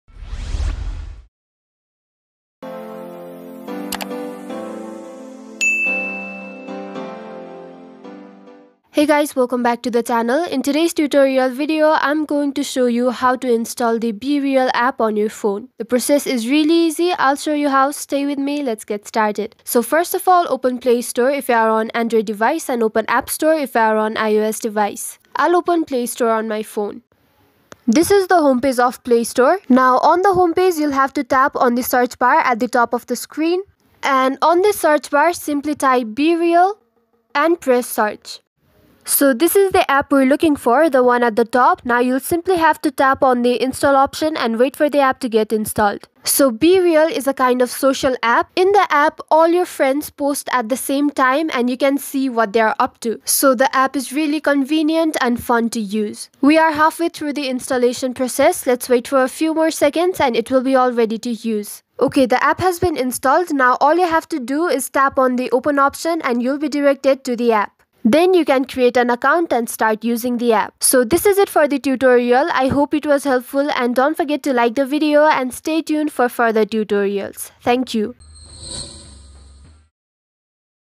How To Download & Install BeReal App | 'BeReal. Your friends for real.' Mobile App Download Guide
These are the steps to Download & Install BeReal App:
Step 1 = Open the Play Store app.
Step 2 = Tap on the search bar at the top of the screen.
Step 3 = Type in bereal and tap on Search.
Step 4 = Then, tap on Install.